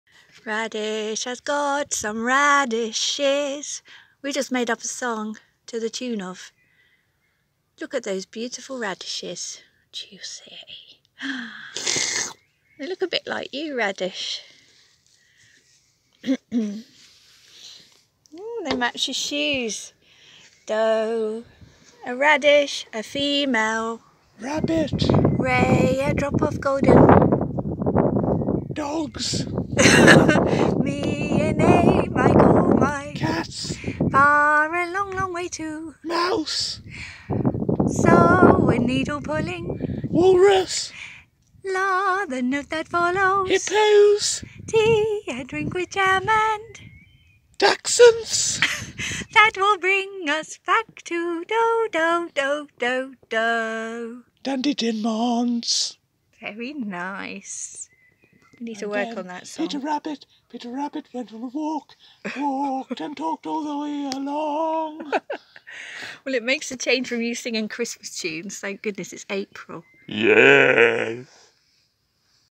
Episode 19. Radish and the radishes
We sing for the joy of Radishes.